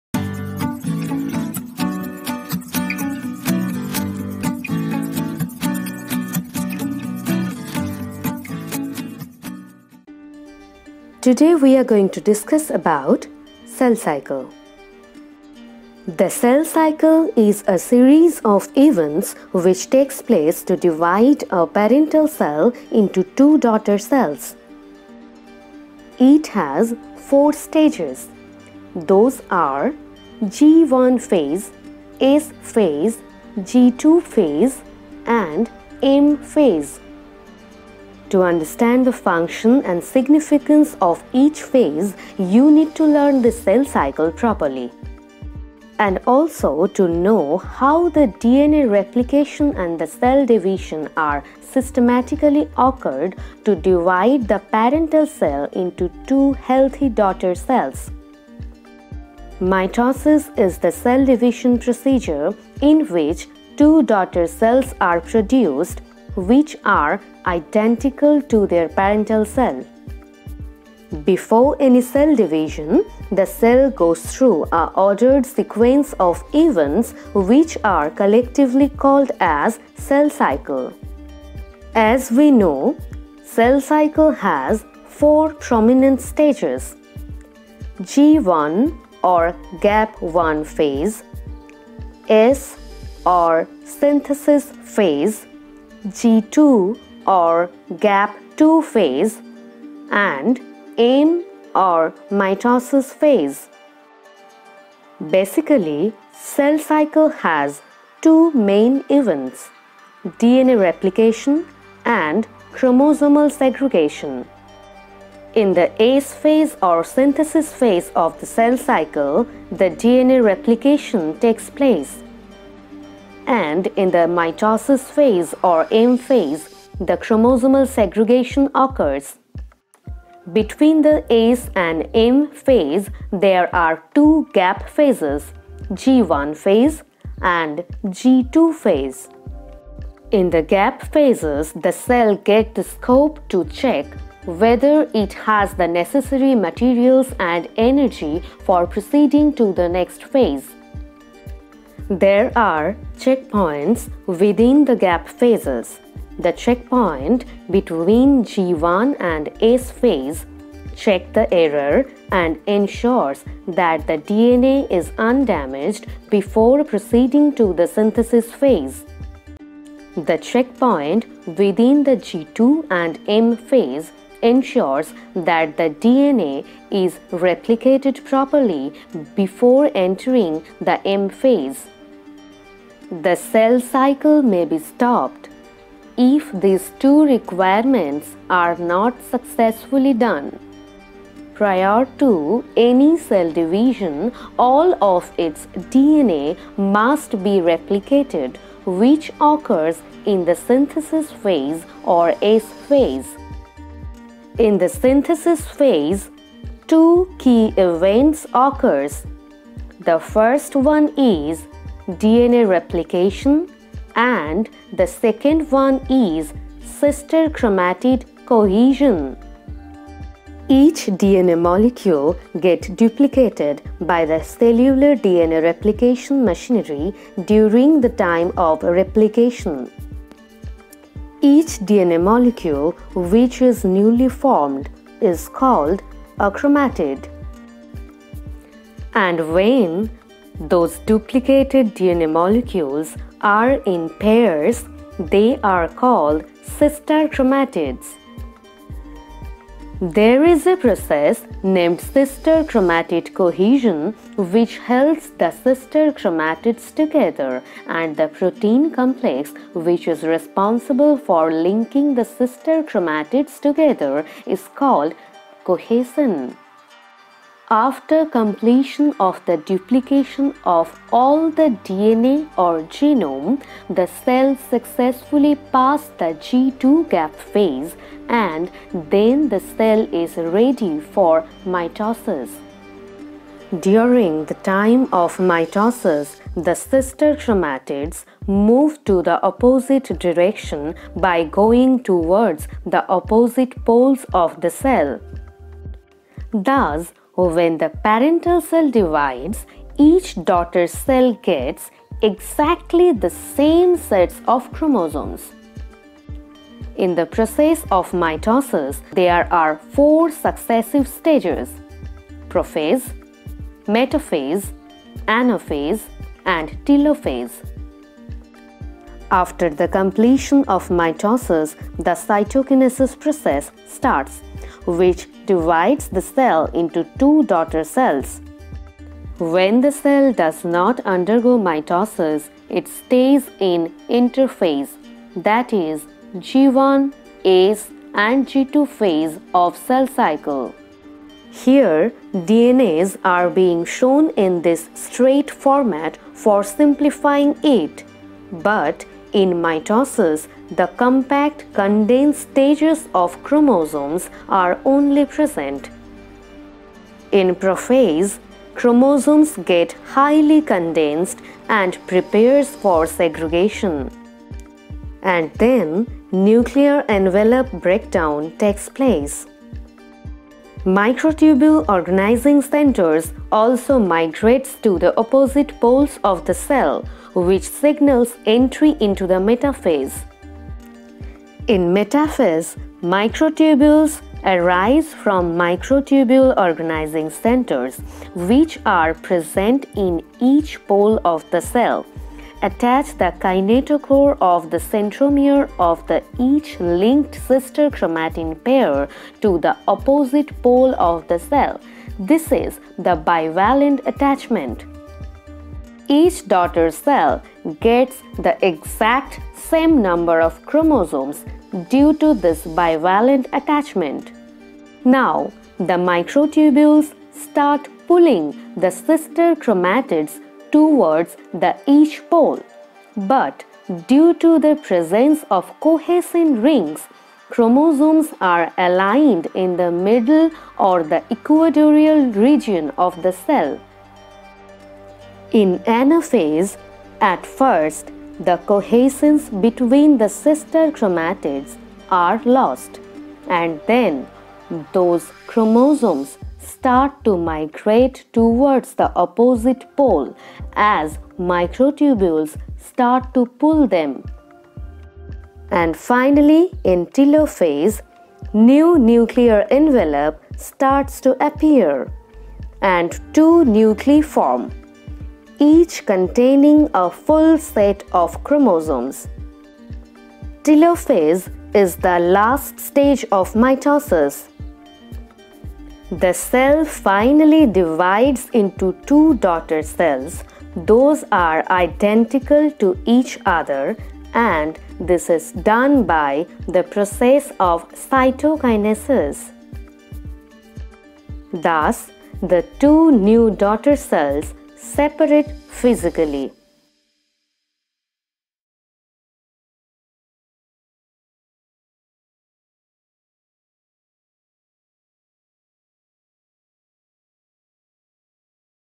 Cell cycle Animation
A detailed animation of the events of the cell cycle